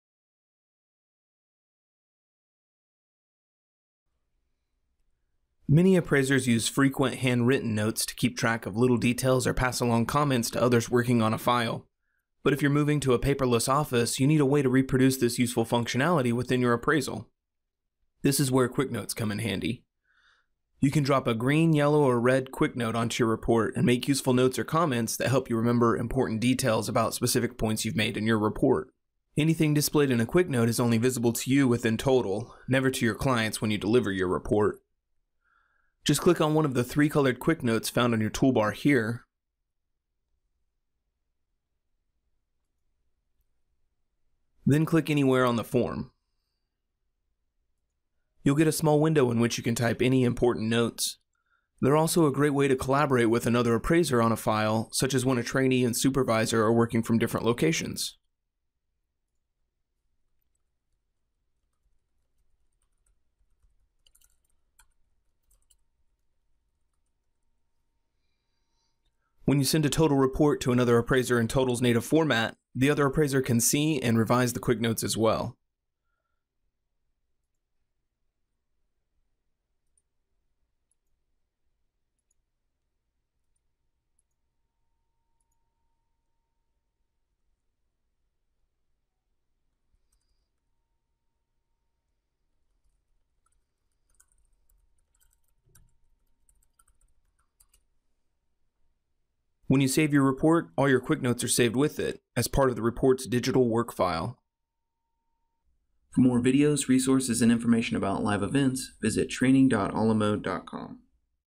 Getting Started: Adding a QuickNote to your report
QuickNotes are a great tool for keeping important "appraiser-only" notes about your assignment, and for communicating back-and-forth with other appraisers you may be collaborating with on a report.  See how to create them in this quick video.